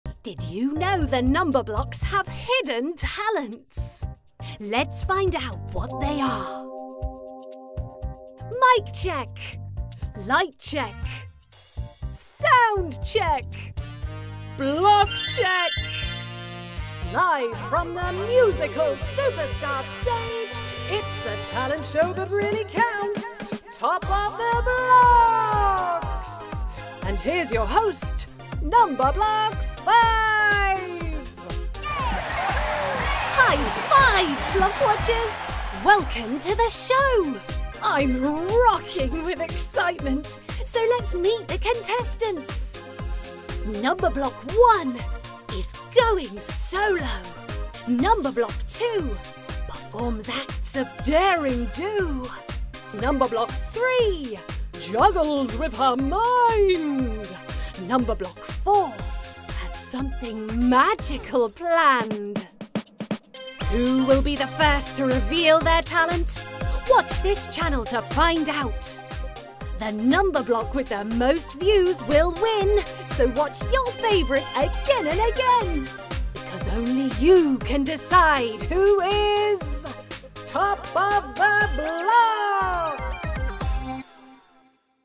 Top of the Blocks - Roll up, roll up for the BIGGEST show! | Toy Play & Count
Get ready for the most exciting event of the year... Top of the Blocks! Who will rise to the challenge and claim the title as the ultimate Numberblock performer? It’s a thrilling competition filled with fun, surprises and plenty of Number Magic to help your little learners. Don’t miss out on the fun - tune in to find out who will be crowned the winner of this year’s Top of the Blocks!